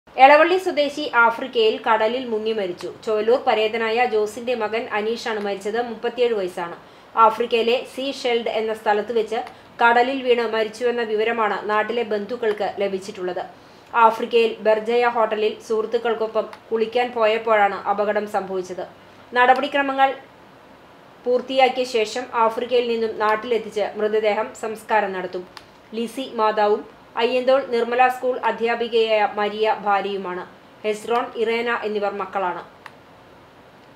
എളവള്ളി സ്വദേശി ആഫ്രിക്കയില്‍ കടലില്‍ മുങ്ങി മരിച്ചു
ചൊവ്വല്ലൂര്‍ പരേതനായ ജോസിന്റെ മകന്‍ അനീഷാണ് (37) മരിച്ചത്. ആഫ്രിക്കയിലെ സി ഷെല്‍ഡ് എന്ന സ്ഥലത്ത് വെച്ച് കടലില്‍ വീണ് മരിച്ചുവെന്ന വിവരമാണ് നാട്ടിലെ ബന്ധുക്കള്‍ക്ക് ലഭിച്ചിട്ടുള്ളത്. ആഫ്രിക്കയില്‍ ബെര്‍ജയ ഹോട്ടലില്‍ സുഹൃത്തുക്കള്‍ക്കൊപ്പം കുളിക്കാന്‍ പോയപ്പോഴാണ് അപകടം സംഭവിച്ചത്.